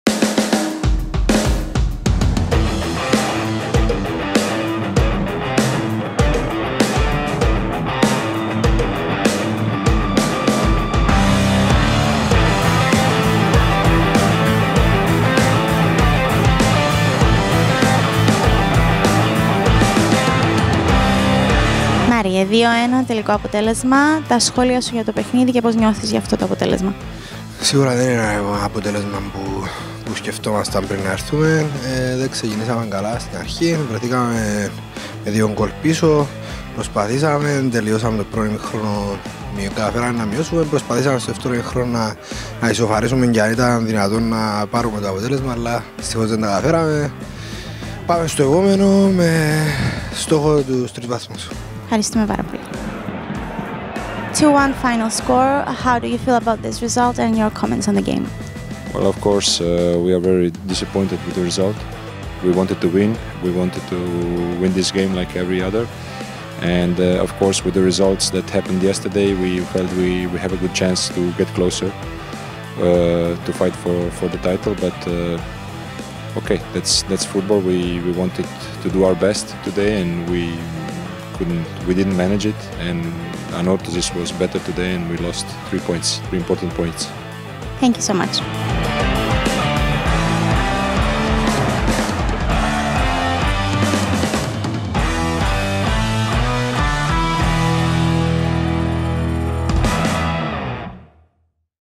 🎤 Post-Match Interview | Anorthosis Famagusta vs Pafos FC: Marios Demetriou and Sandro Bloudek
🗣️Post-Match Interview with Marios Demetriou and Sandro Bloudek